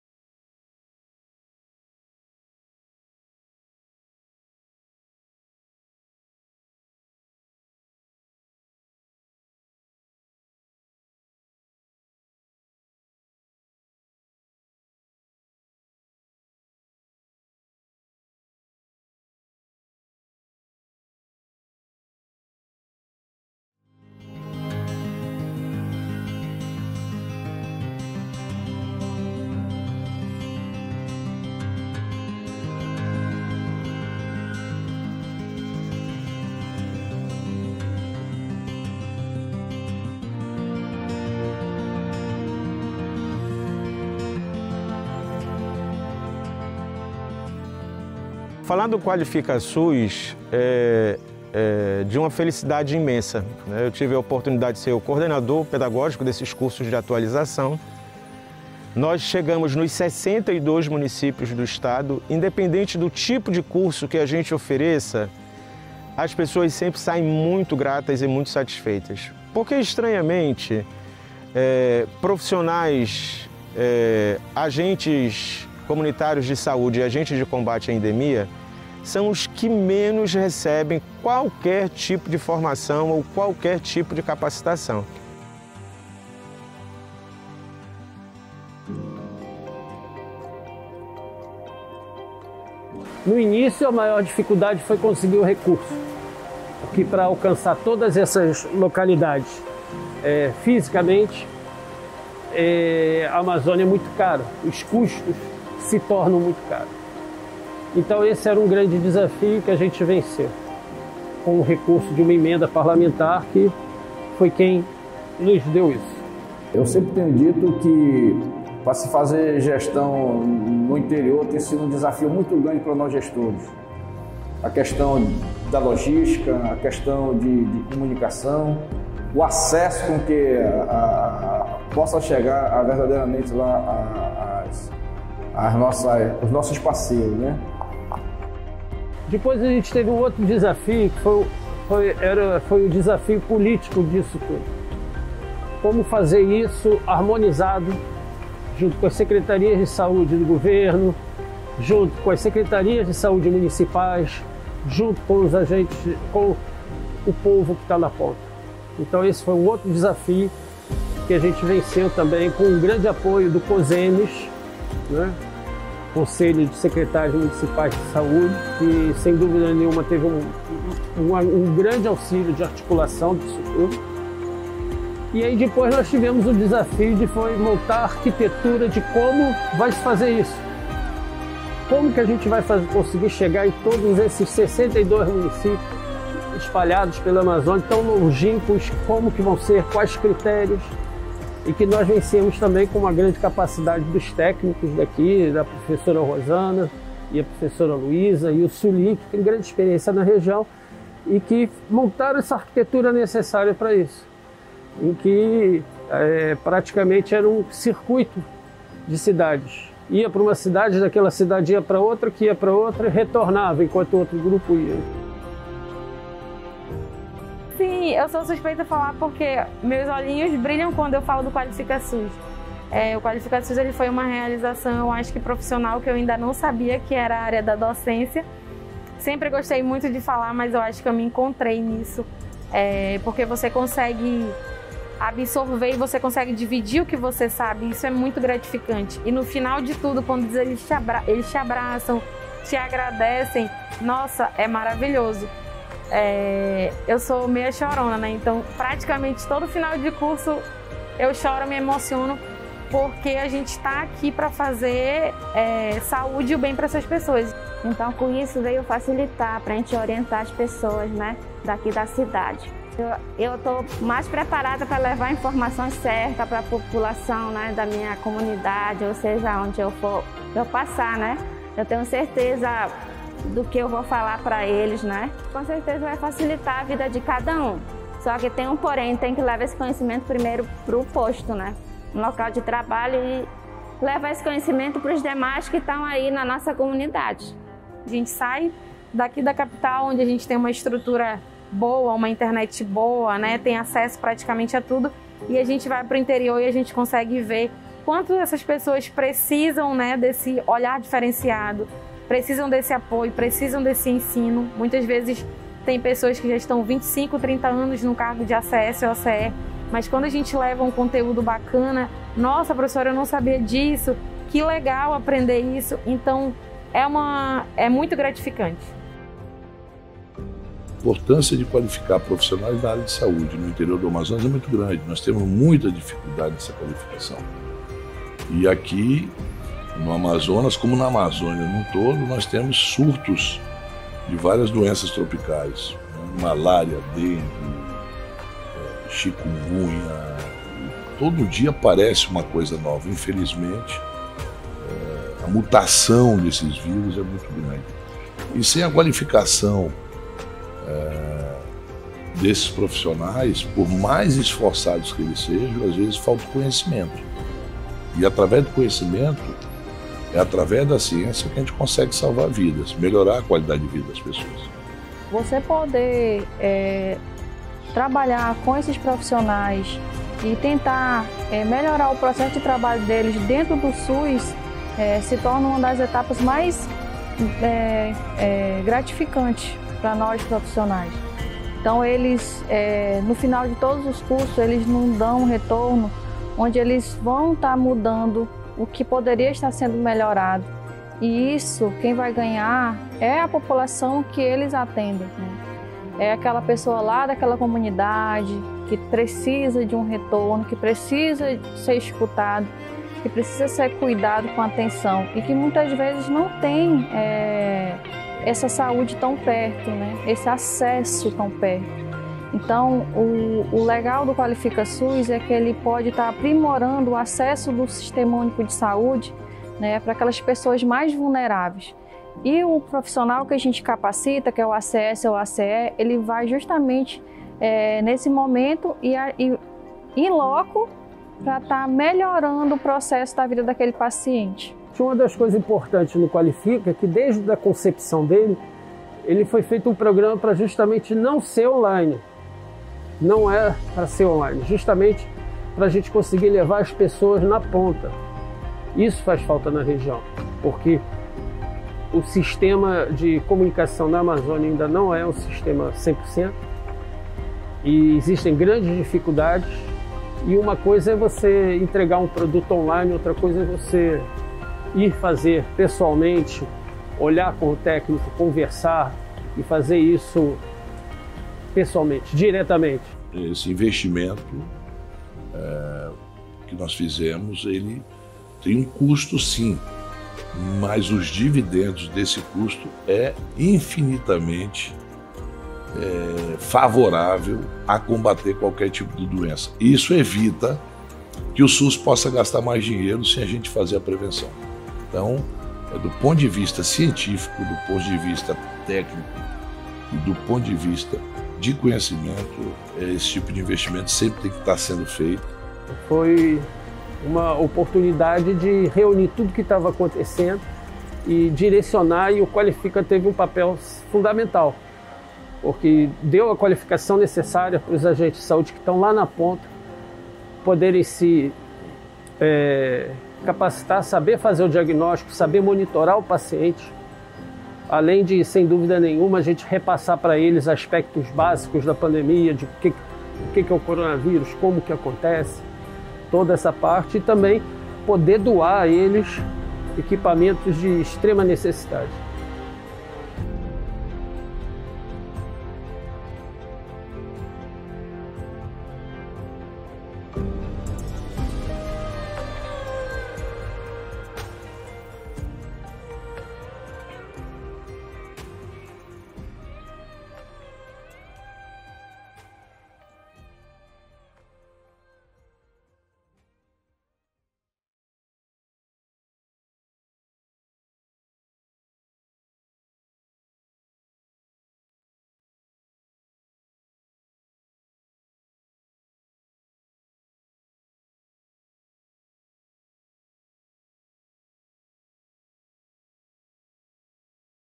QualificaSUS - Construindo histórias
Este vídeo apresenta as atividades desenvolvidas em cursos de atualização ofertados, no âmbito do Projeto QualificaSUS, aos Agentes Comunitários de Saúde - ACS e Agentes de Combate à Endemias - ACE, nos 62 municípios do estado do Amazonas, entre os anos de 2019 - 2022.